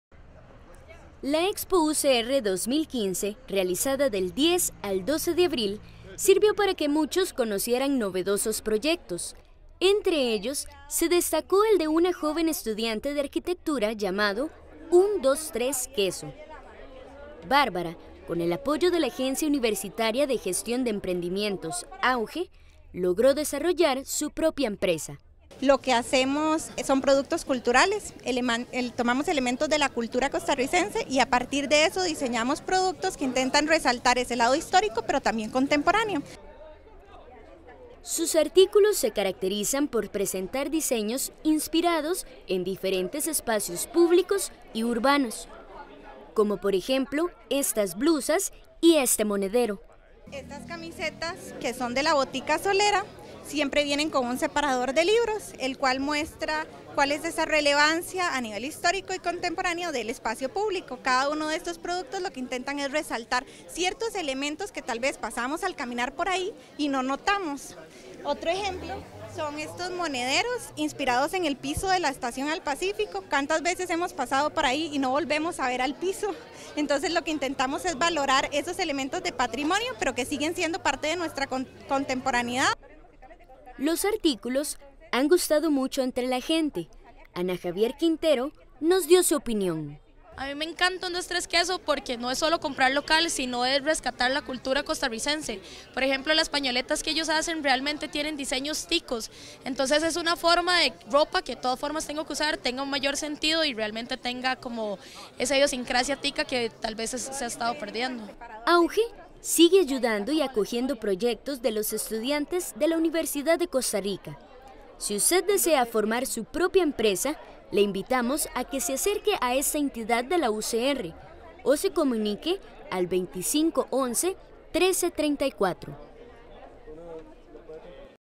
Proyecto Auge
¿Se imagina traer encima la Estación del Pacífico o el mercado central de San José? Esto es posible a través del proyecto de una estudiante de la Universidad de Costa Rica.